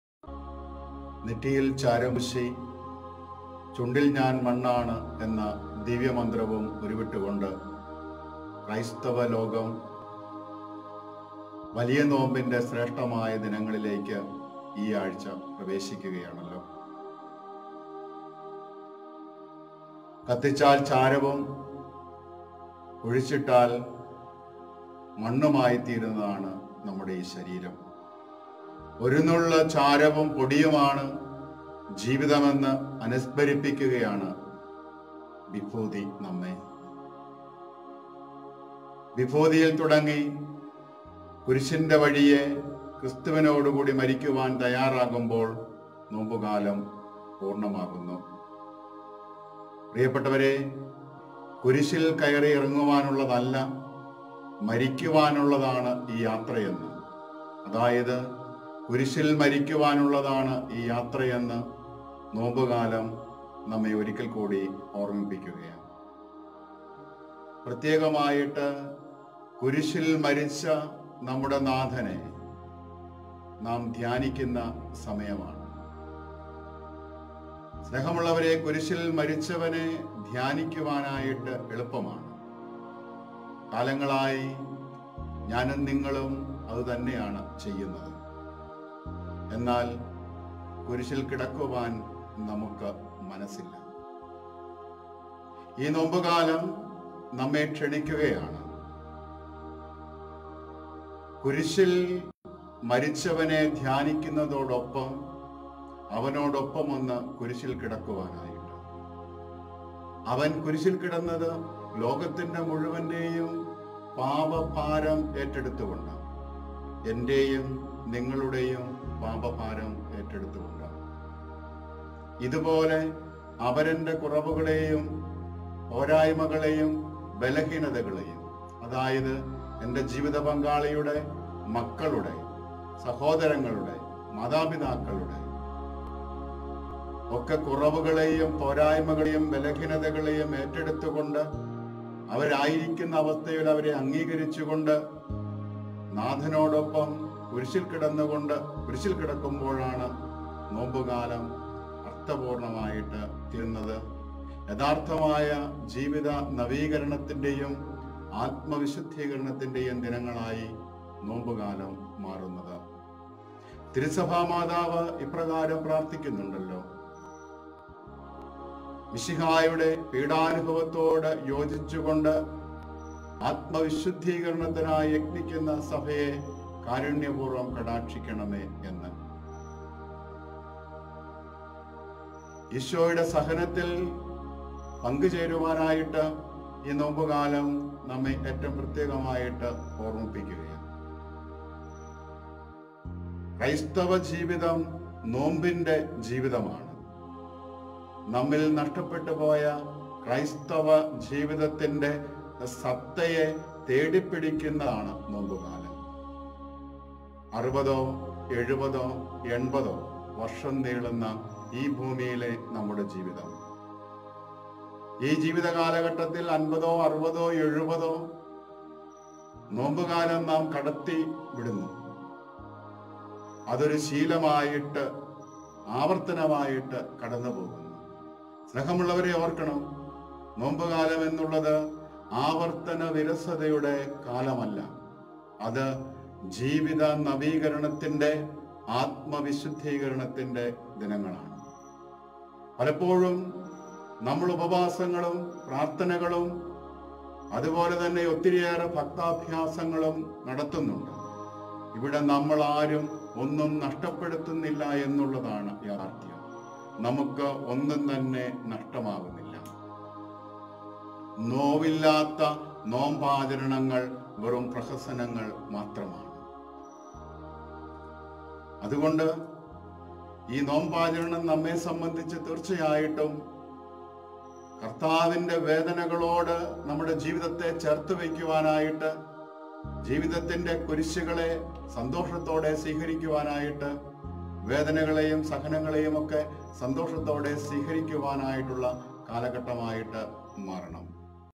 വിഭൂതിയിൽ തുടങ്ങികുരിശിൻ്റെവഴിയേ ക്രിസ്തുവിനോട് കൂടി മരിക്കാൻ തയ്യാറാകുമ്പോൾനോമ്പുകാലംപൂർണ്ണമാകുന്നു
ഡയറക്ടർ
ബെനഡിക്ടൈൻ ധ്യാനകേന്ദ്രം
എയ്ഞ്ചൽവാലി, കുമളി, ഇടുക്കി